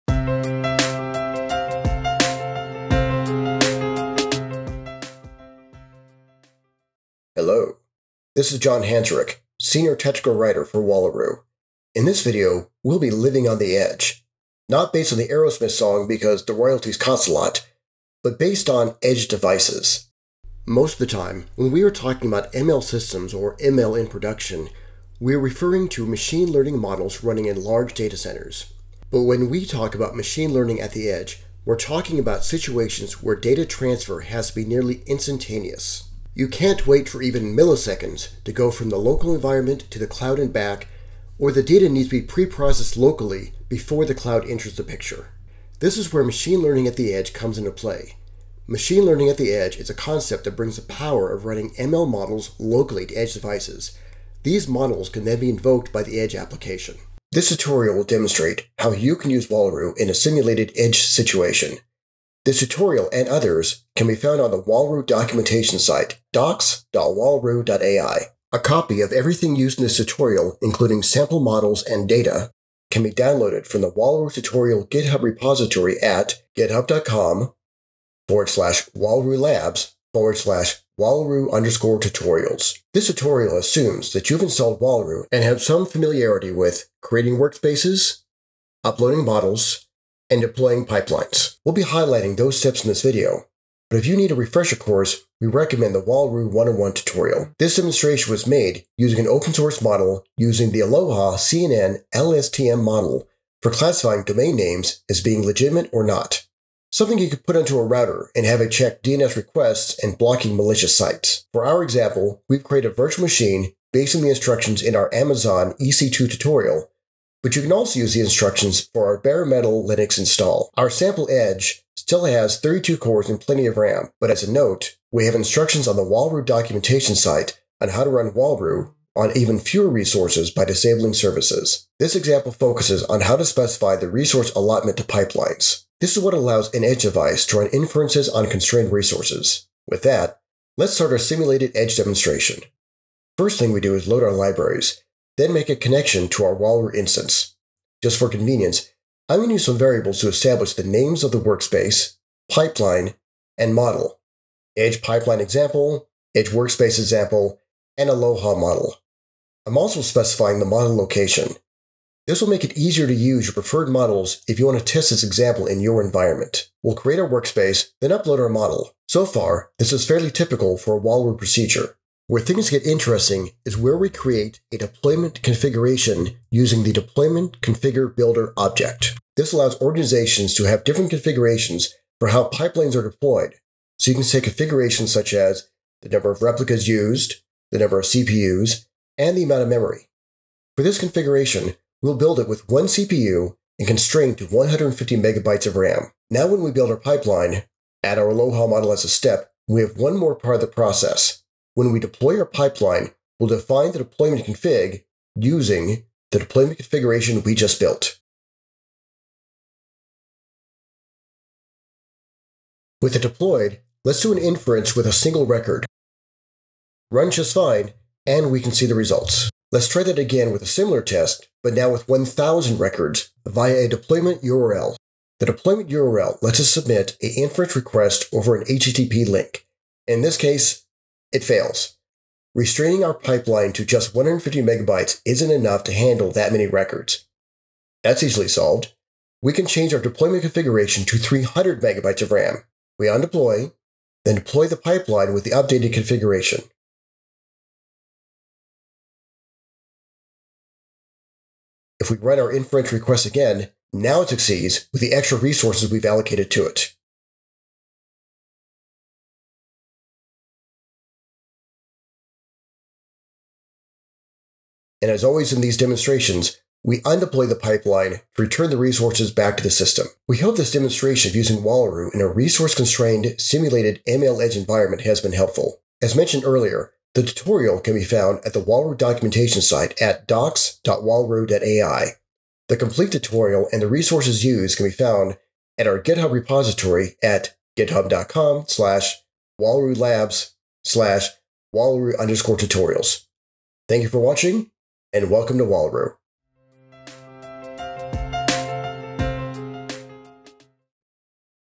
Deploying ML to the Edge with Wallaroo Tutorial
This tutorial walks through a simulated ML Edge deployment scenario using the Wallaroo platform.